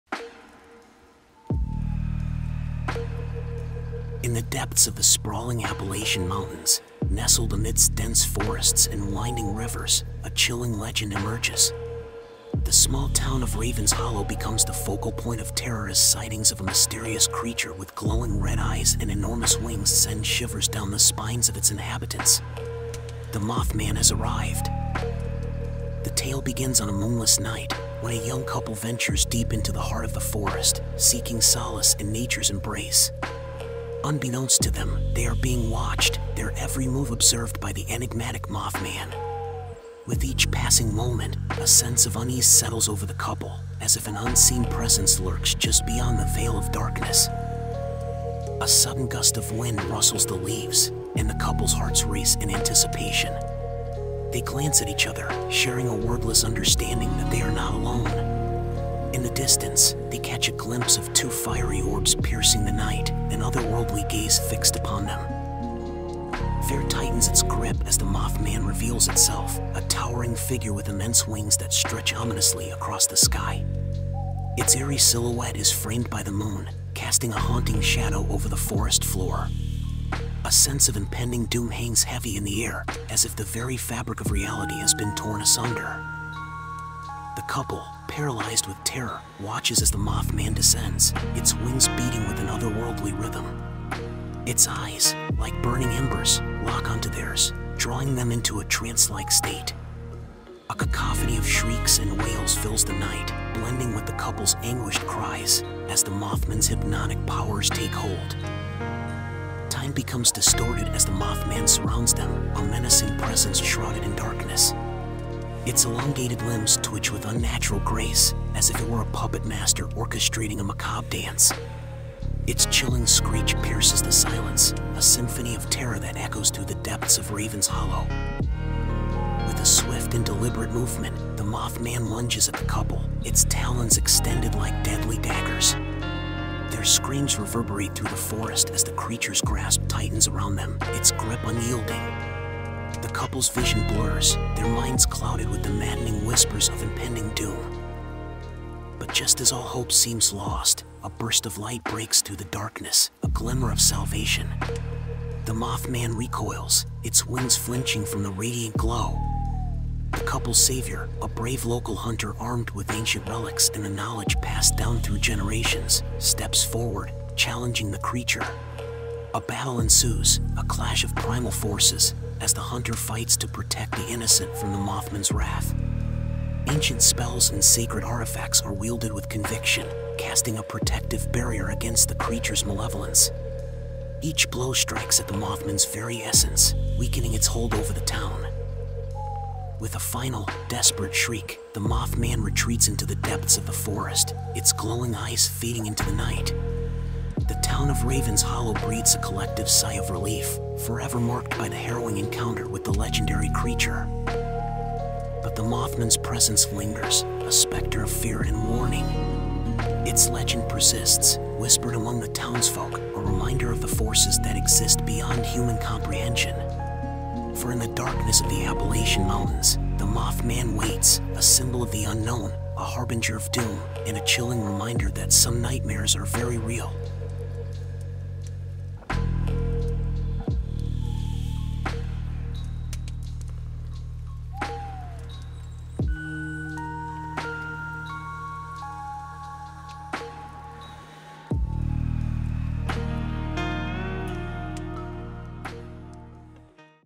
Scary Story - The Enigma of Raven's Hollow: The Mothman's Curse
In the heart of the Appalachian Mountains, the town of Raven's Hollow becomes a breeding ground for fear and paranoia as the legendary Mothman emerges from the shadows. With its piercing red eyes and massive wings, the creature haunts the dreams of the townspeople, leaving them paralyzed with terror. Brace yourself for a journey that will leave you questioning the limits of human understanding and the existence of unimaginable creatures that dwell in the depths of the Appalachian wilderness.